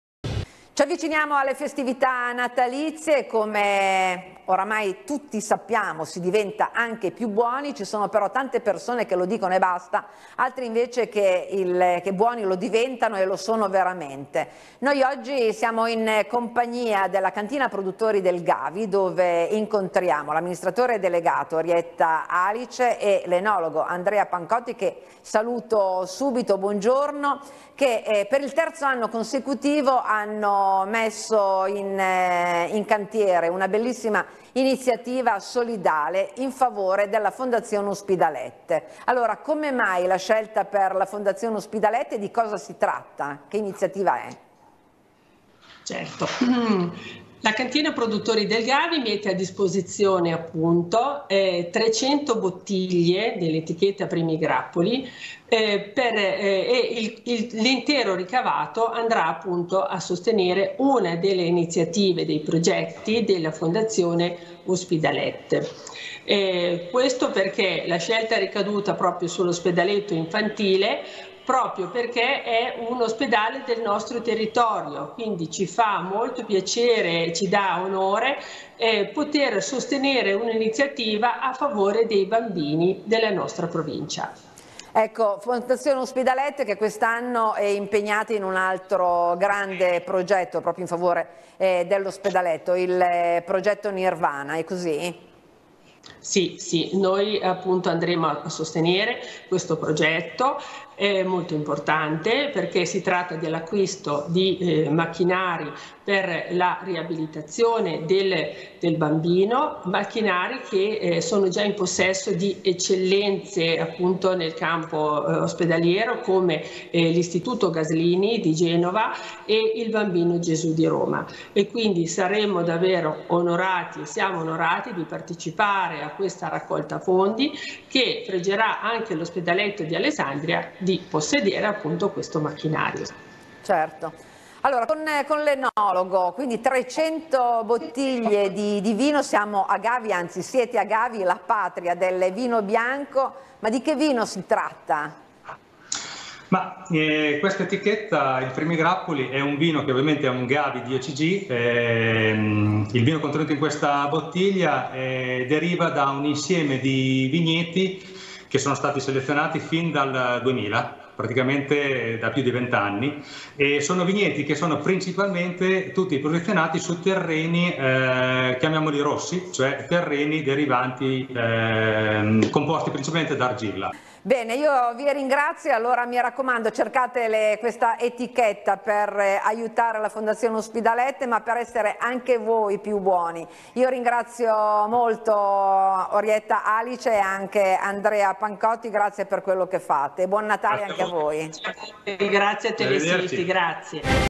15/12/22 - Bottiglie solidali: la Cantina Produttori del Gavi in favore della Fondazione Uspidalet
15/12/22 - Bottiglie solidali: la Cantina Produttori del Gavi in favore della Fondazione Uspidalet